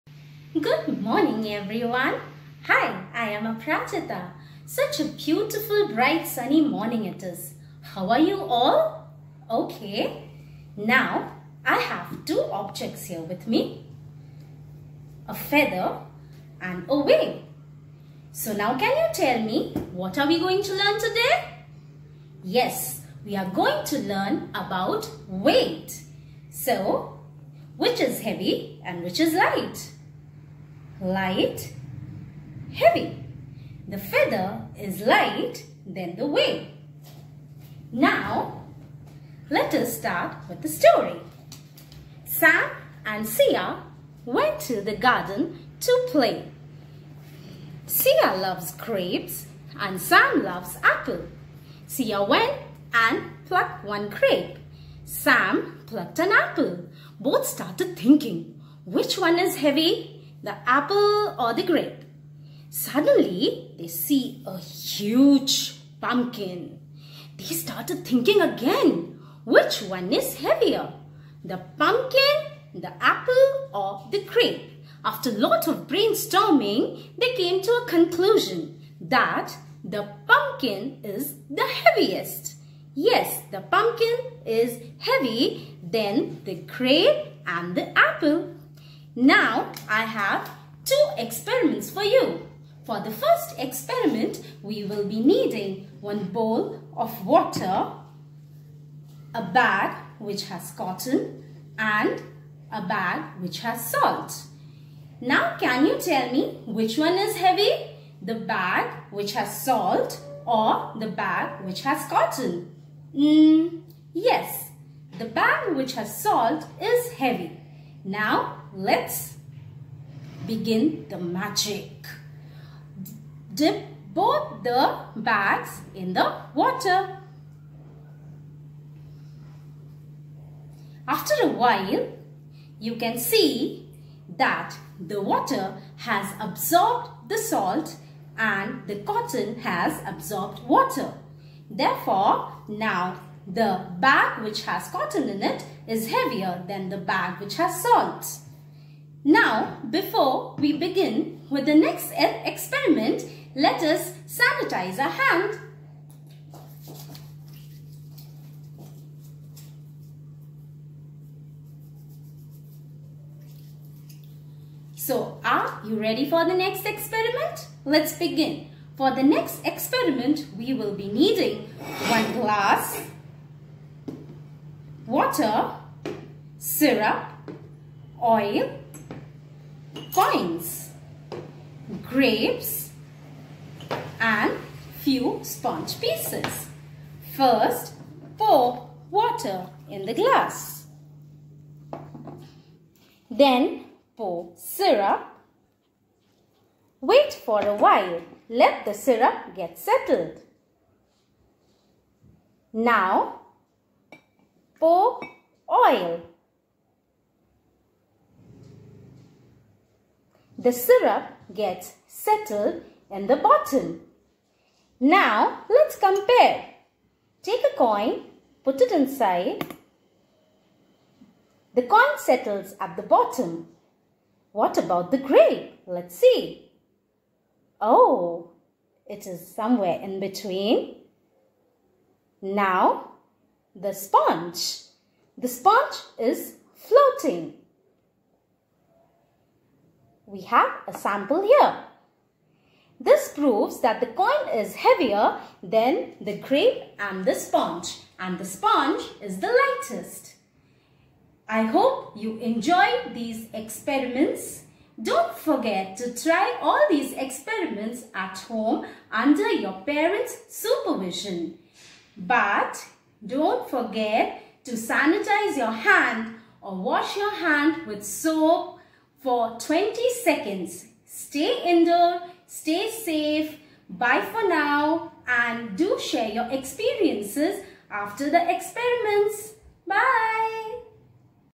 Fun with Measurement - Weights
Day 8 of Lockdown

Our Grade 1 teacher Ms.Aparajita demonstrating an experiment on the different types of weights.The experiment helps to develop the ability to make comparisons of objects in terms of their weight.

These videos are developed to keep the children engaged during the covid-19 lockdown period.